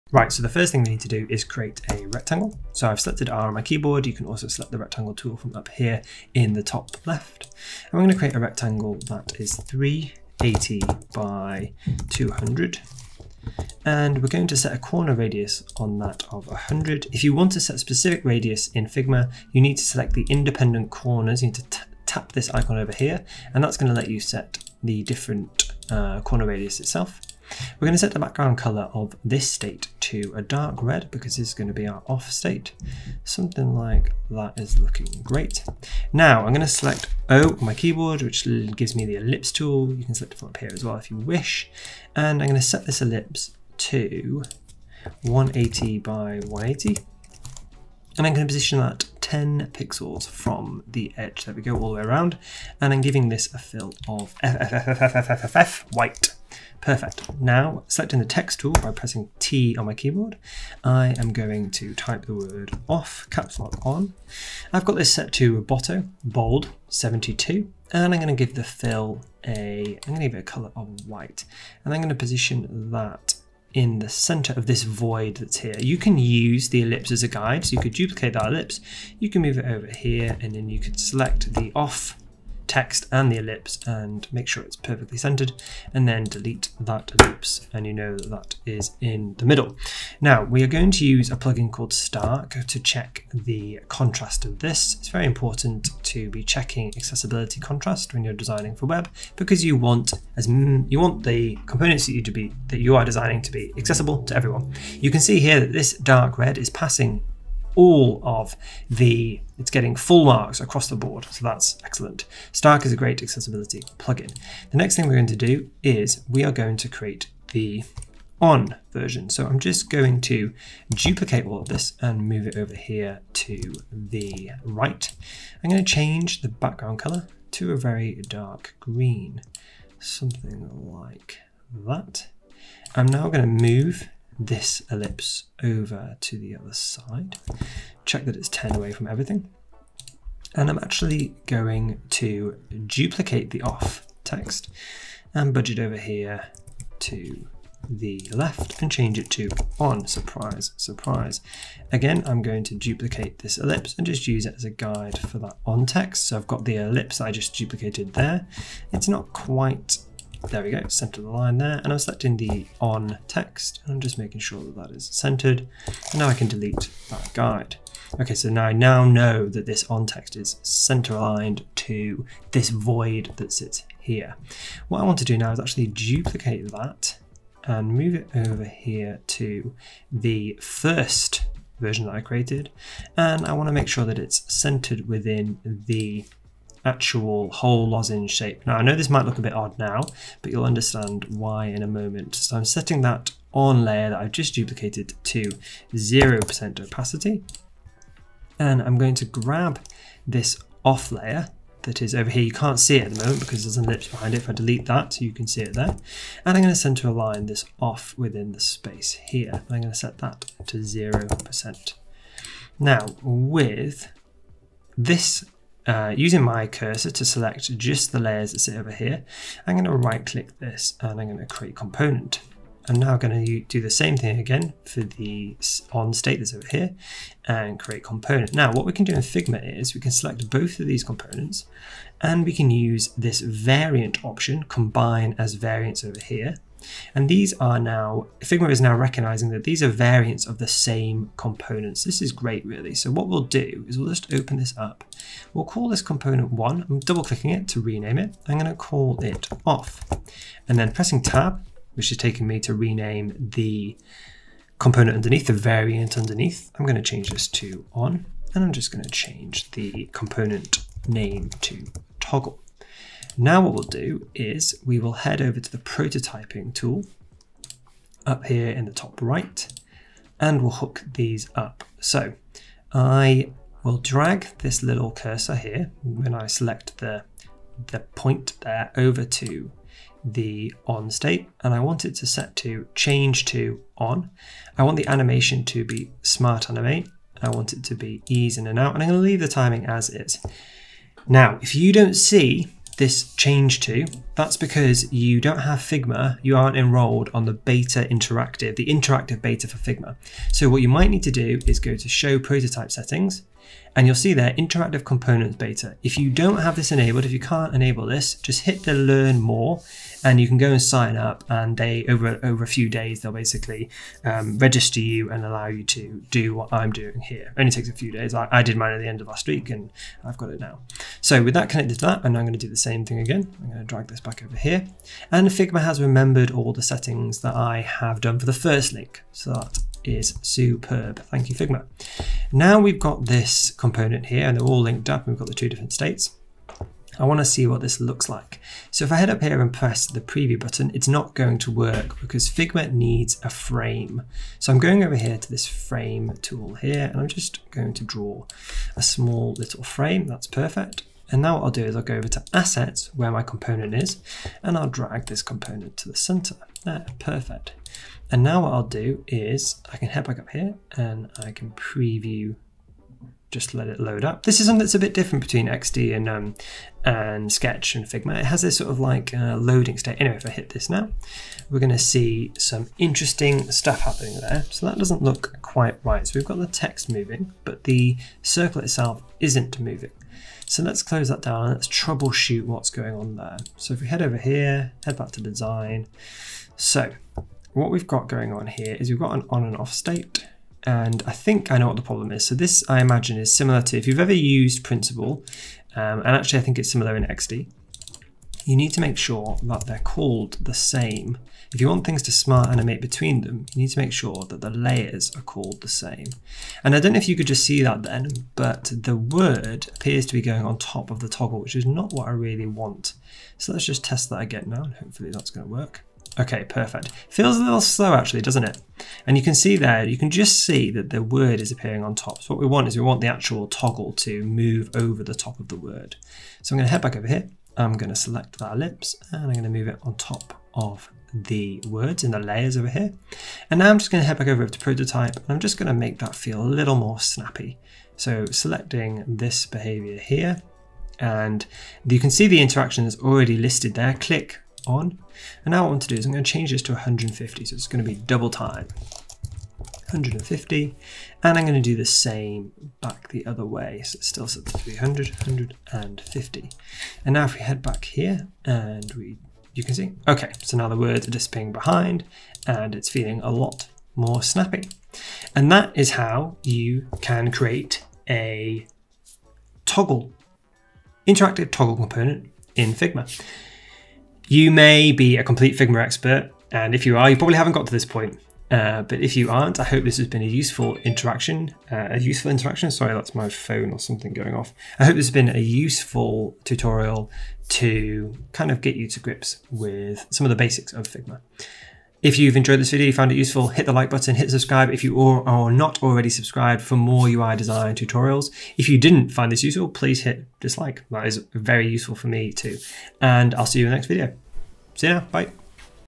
Figma Tutorial: Create an interactive toggle switch
In this UI design tutorial, I'll show you how to create an interactive toggle component in Figma. If you're new to Figma, as I am, hopefully this video will help you to find your feet.

The toggle component is very common, typically found in apps but also pretty prolific on the web these days.

I'm just having a play around with Figma, if you found this useful and want more tutorials let me know by hitting the 'Like' button or leaving a comment.

Top Adobe XD tutorials